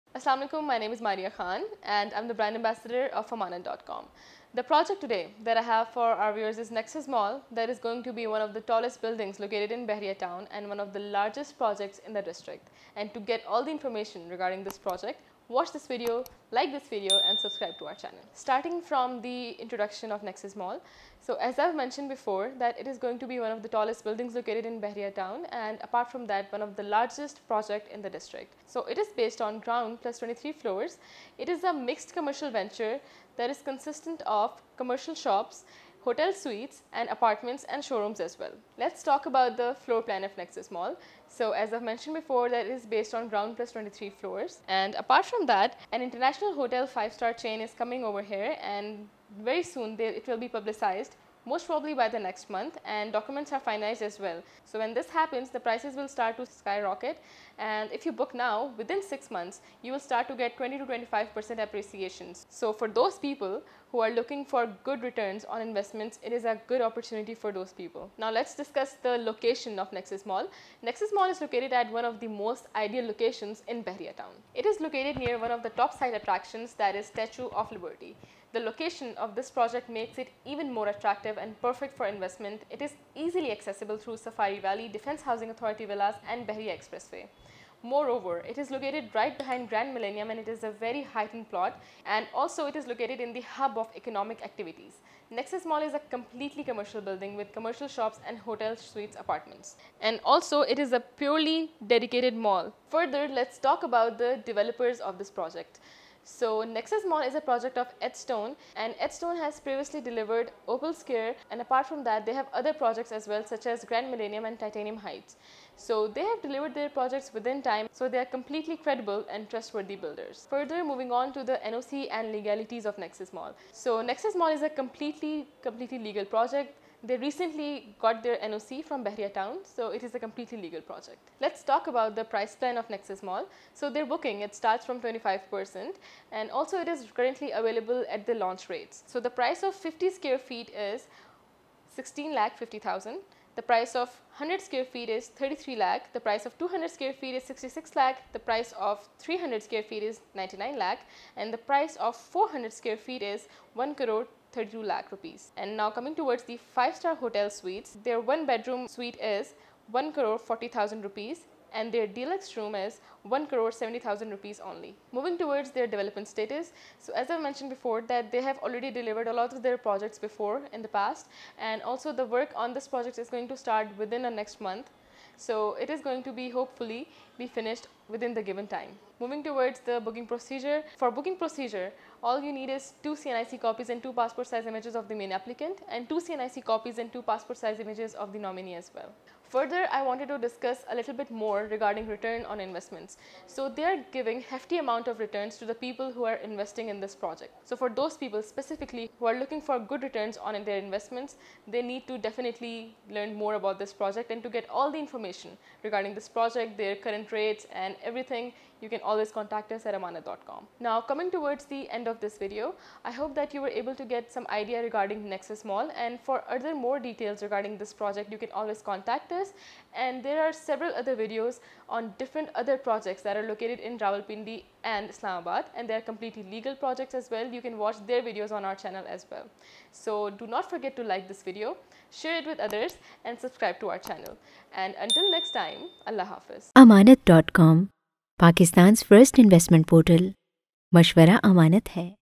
NEXUS MALL Bahria Town || complete overview in English by Ammanat.com
Nexus Mall a high-Rise Project, right in the heart of Bahria Town. It would not sound wrong if we state that it is going to be the tallest tower in Bahria Town, Islamabad.
The mall will be covering a lot of aspects such as luxury shops, Comfort living spaces, Cooperate offices, and a grand Shopping Complex. Mall features cover almost 12.6 kanals of commercial land in Bahria Town. Its Futuristic design will bring innovation to the construction industry across the country.For more details, heads over to the video.